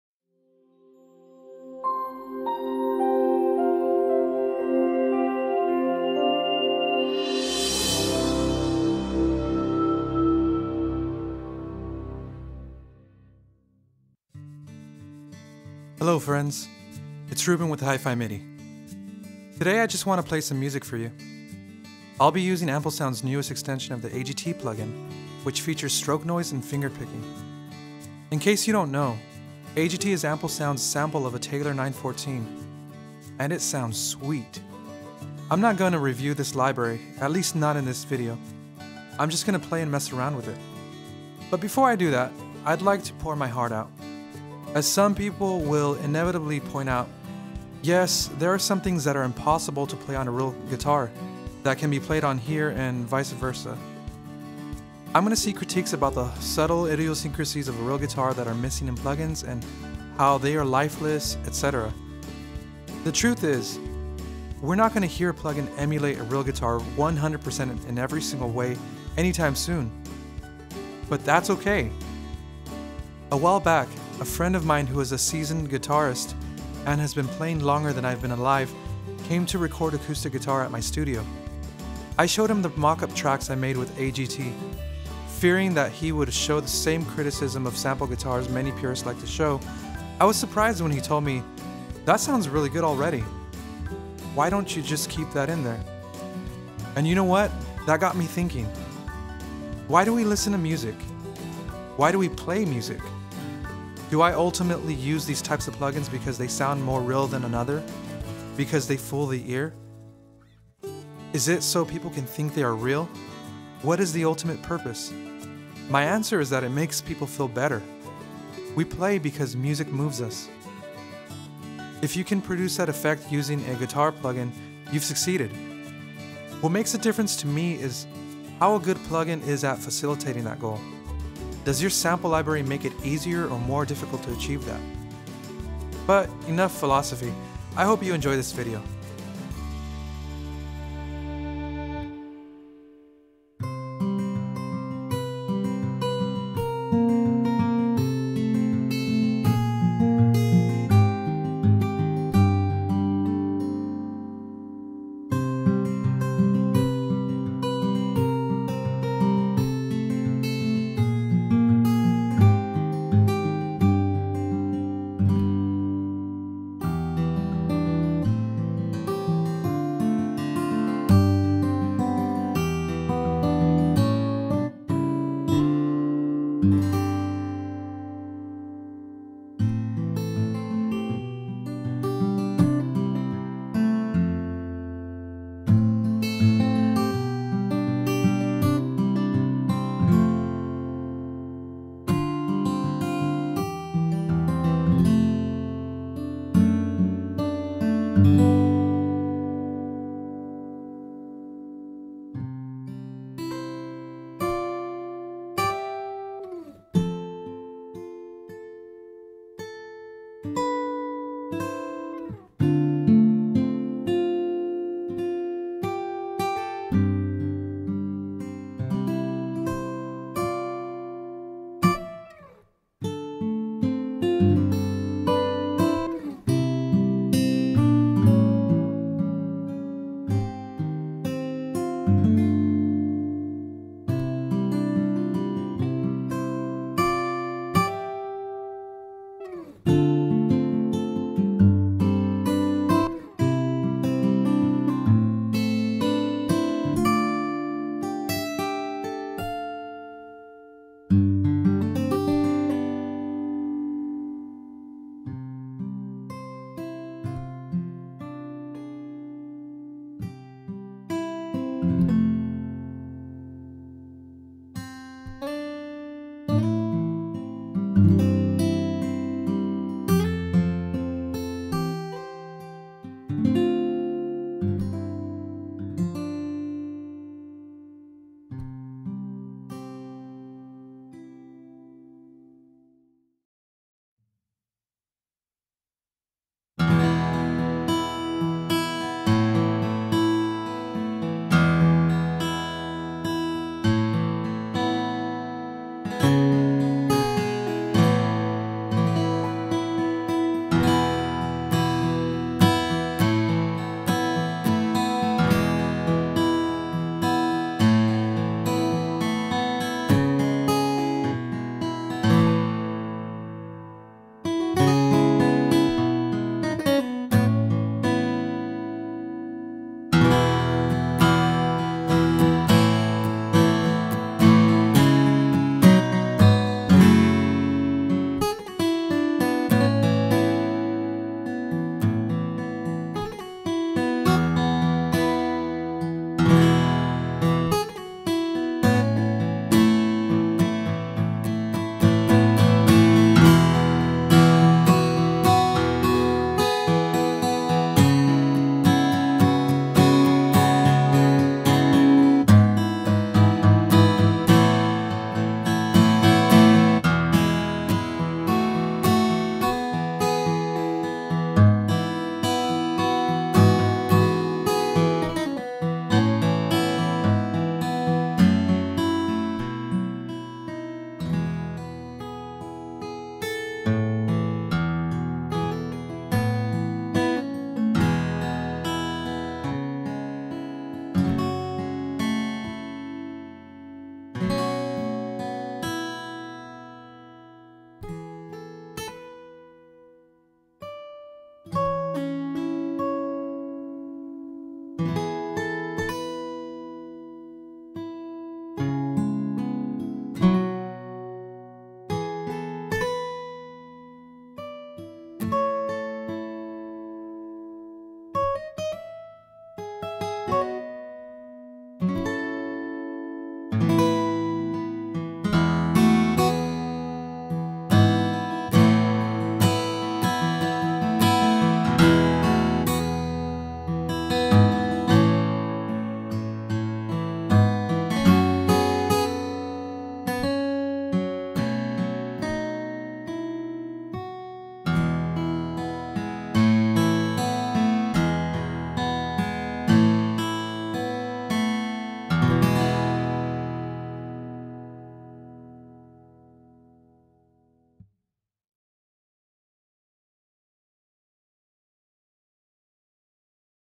AmpleSound AGT II Extension (Taylor 714CE VST)
A short improvised demo of AmpleSound’s AGT II with extension.